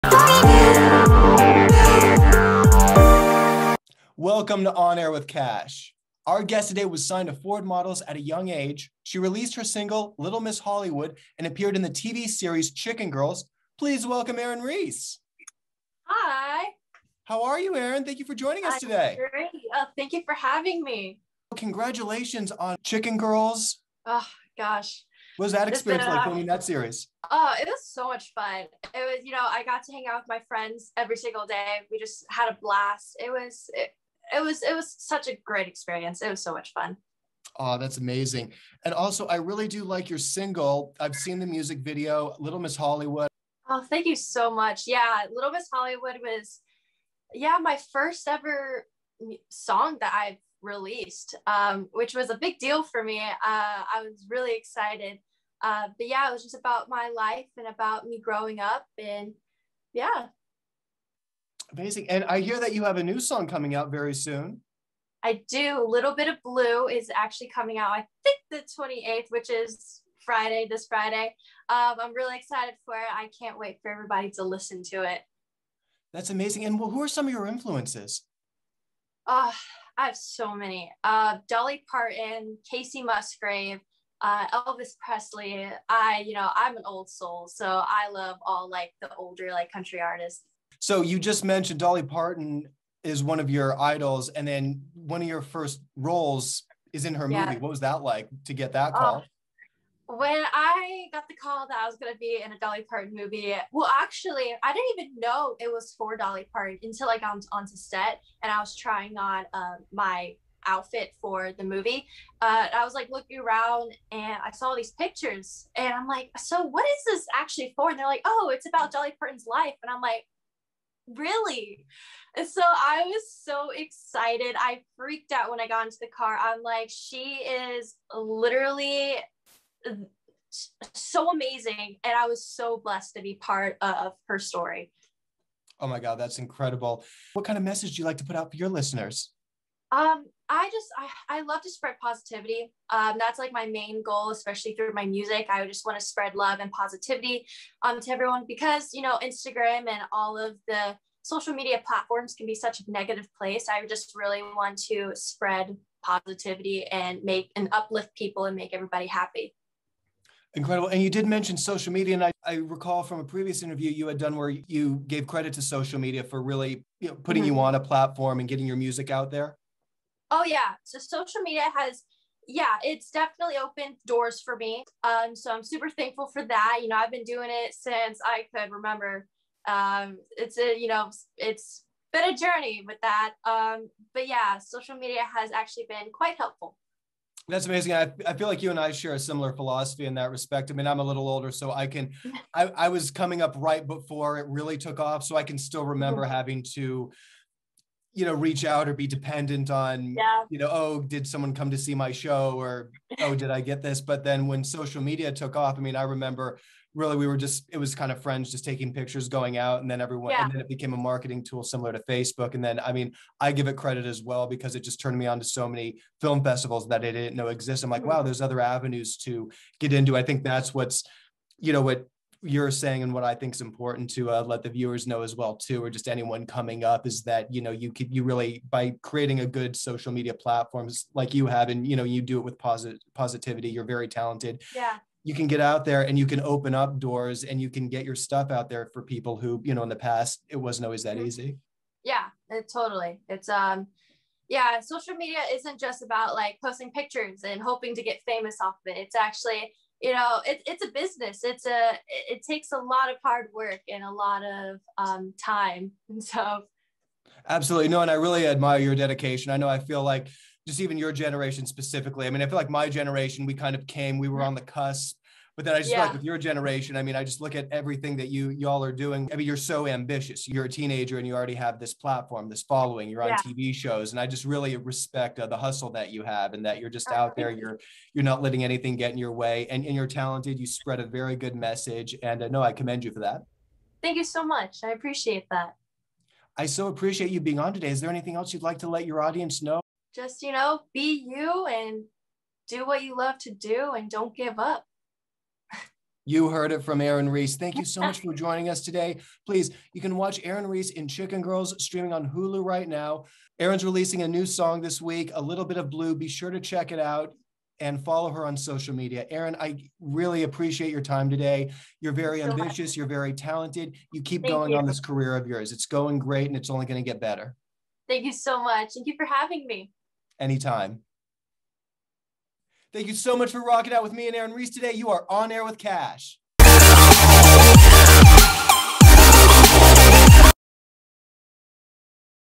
"On Air with Ka$h" Erin Reese (Chicken Girls) Interview - Episode 28
Host Kash Hovey interviews Erin Reese about her career. A Florida native Erin was discovered at an early age by Ford Models in Miami Beach, FL. Erin was always comfortable in front of the camera and has graced many magazines as a model, however, at age seven she began to audition for commercials booking with Party City and Disney to name a few. If acting and modeling were not enough for this young girl Erin soon found a passion for dance. At age seven she began participating as a competitive dancer with her local dance school and team. Learning all forms of dance Erin has continued her training yet no longer competes. Erin was introduced by her Florida agent to Adrian R'Mante the founder of CGTV actors program. Adrian is a working actor well known for his role in Disney's Suite LIfe of Zack & Cody. Erin trained and completed the CGTV graduate program which allowed her the opportunity to showcase her talent for top agents and managers in Los Angeles, CA. Erin is currently represented by agent Milton Perea of AEFH. She has performed in several local musical theater performances using her dancing and acting skills. During this time, Erin was encouraged to begin vocal lessons and is now working with one of LA's best vocal coaches, music producer, composer and singer Griffith Frank. Erin has worked with music producers in Los Angeles recording cover songs, as well as, releasing two original songs. Erin is a well rounded performer taking pride in all facets of training. When not modeling, dancing and recording Erin was able to fit in two consecutive summer's filming NBC/Universal's very popular made for television movies Dolly Parton's Coat of Many Colors and Christmas of Many Colors: Circle of Love. Erin is working to build her social media presence and currently on the hit teen YouTube show Chicken Girls. Erin has received many compliments on and off set as to her work ethic, kindness and ease in taking direction well. Living the bi-coastal life Erin travels to where her next booked job or audition takes her.